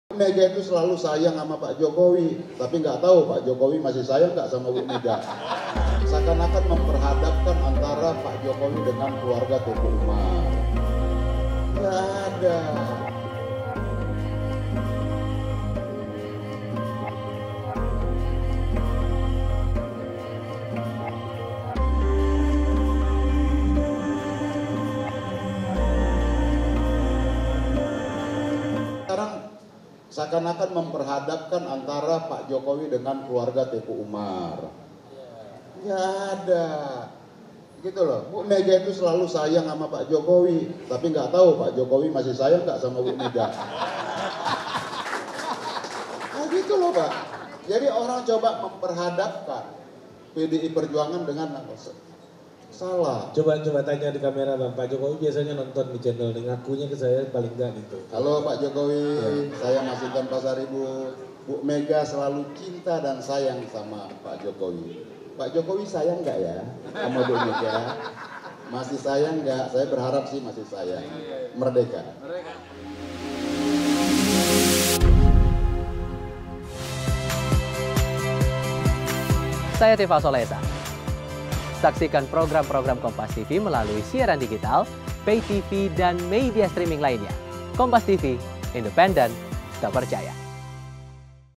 Masinton Soal Isu Keretakan Hubungan Jokowi dan Megawati : Ibu Mega Menyayangi Jokowi
KOMPAS.TV - Masinton Soal Isu Keretakan Hubungan Jokowi dan Megawati : Ibu Mega Menyayangi Jokowi